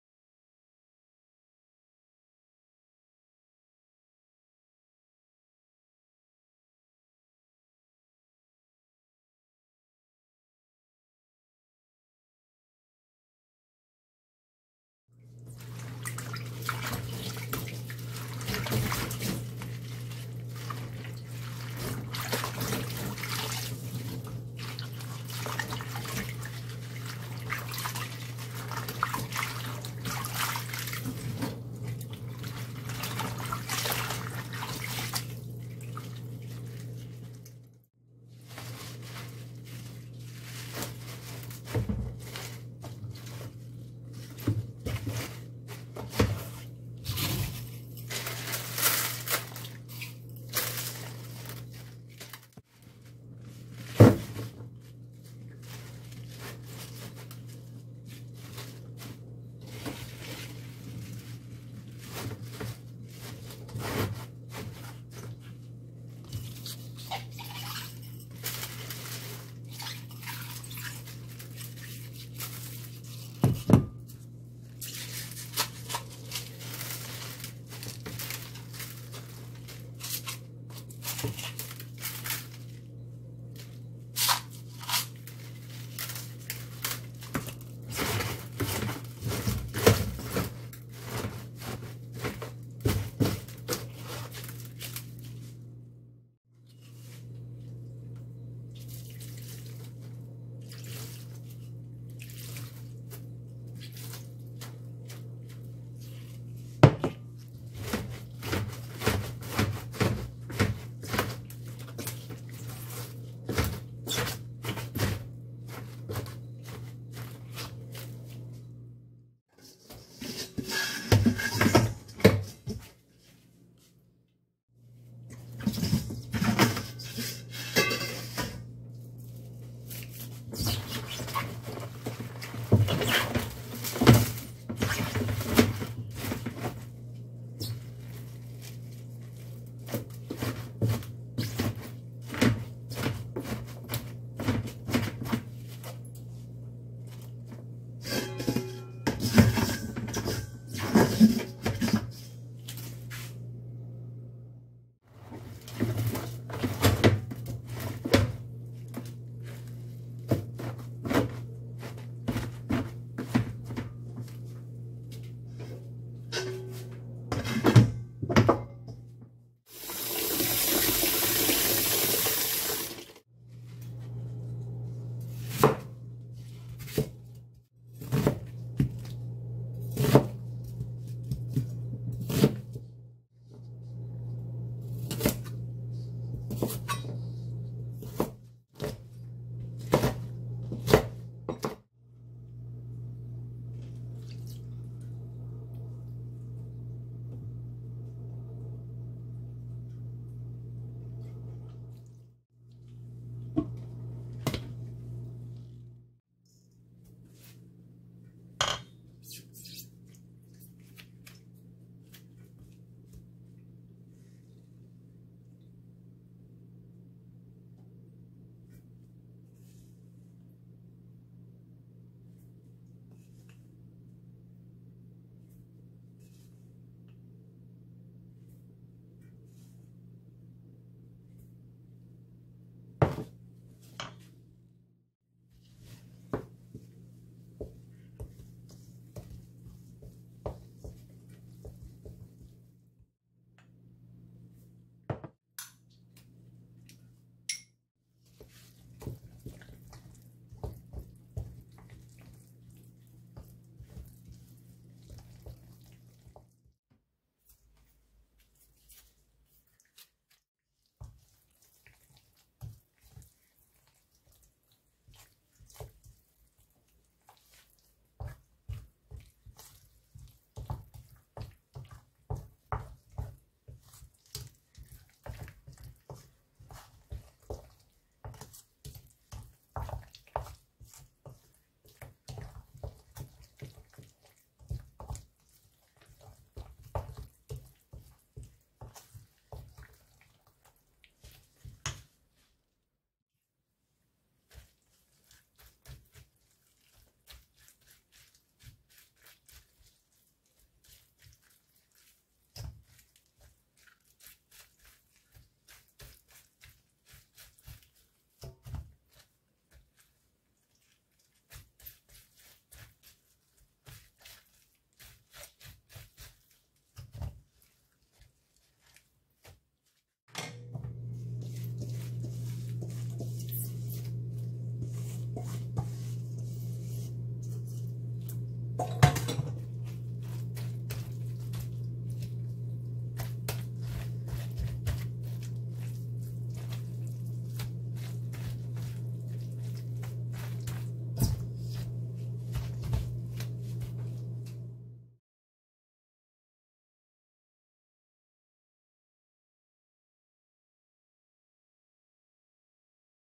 설탕없이도 달고 꼬소한 얼갈이 배추김치
채소를
갈아넣어
설탕없이
만들어도
단맛이
충분한
'얼갈이배추김치'